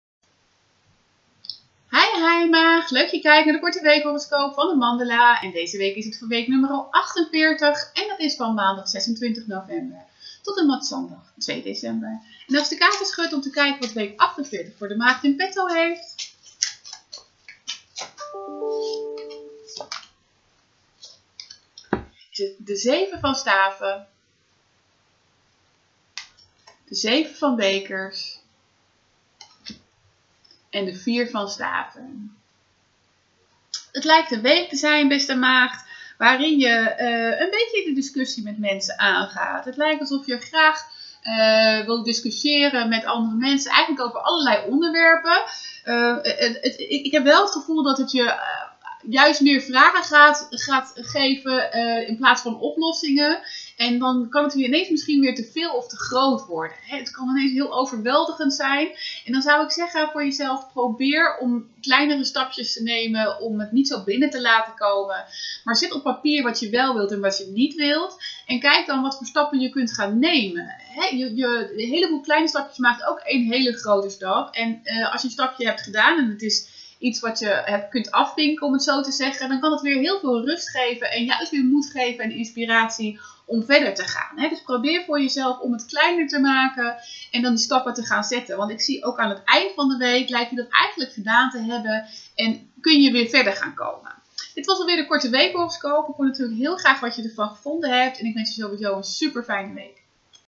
Weekhoroscoop Maagd week 48 2018
Korte tarot weekhoroscoop Maagd week 48 - maandag 26 november 2018 t/m zondag 2 december 2018.

Decemberaanbieding: jaarlegging 2019 op video, mail, facebookmessenger of in een persoonlijk consult 25 euro! Ook leuk om cadeau te geven.